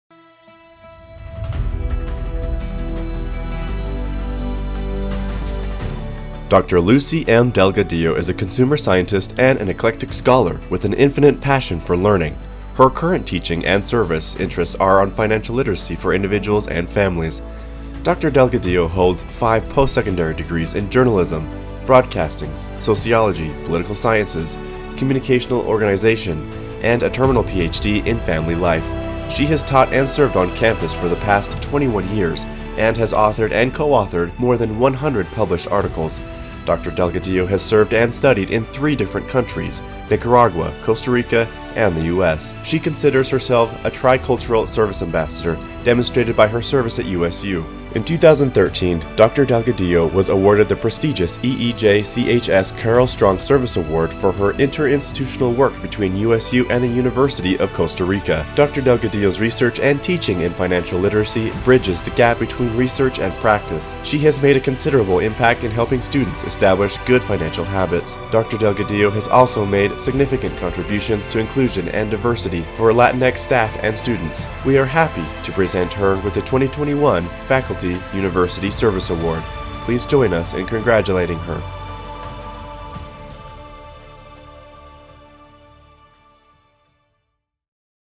Faculty University Service Award - Lucy Delgadillo
Dr. Lucy M. Delgadillo is a consumer scientist and an eclectic scholar with an infinite passion for learning. Her current teaching and service interests are on financial literacy for individuals and families. Dr. Delgadillo holds five post-secondary degrees in Journalism, Broadcasting, Sociology, Political Sciences, Communicational Organization, and a terminal Ph.D in Family Life. She has taught and served on campus for the past 21 years and has authored and co-authored more than 100 published articles.